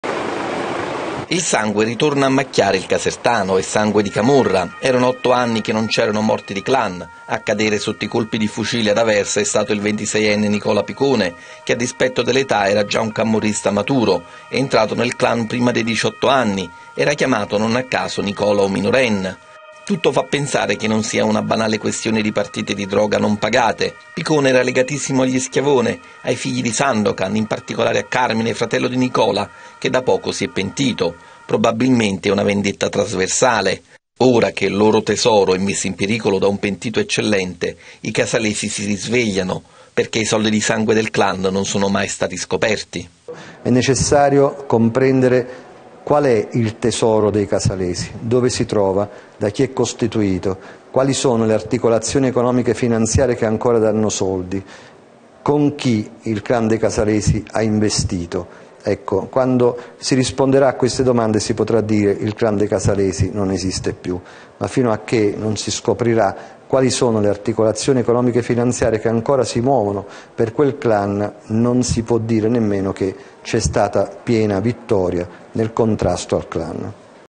Omicidio Caserta, si riapre faida Casalesi. Cafiero de Raho: dov'è il tesoro del clan?
In Campania, dopo il pentimento del rampollo di casa Schiavone, Nicola, la camorra casalese è tornata a sparare e ha ucciso una giovane leva del clan per lanciargli un avvertimento trasversale. Cercano di proteggere il tesoro del clan, mai trovato. Come spiega il Procuratore Nazionale Antimafia Cafiero de Raho. Servizio di Luigi Ferraiuolo